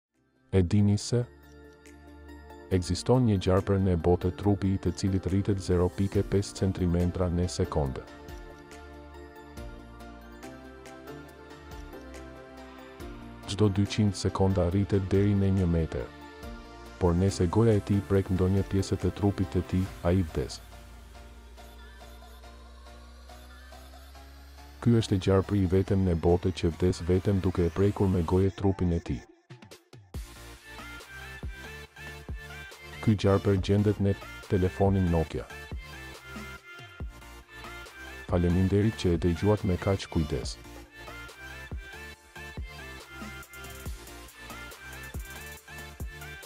Ky gjarper rritet 0.5 cm ne sekonde, per 200 sekonda kap 1 meter shqiperia jone
Ky gjarper rritet 0 5 cm ne sekonde, per 200 sekonda kap 1 meter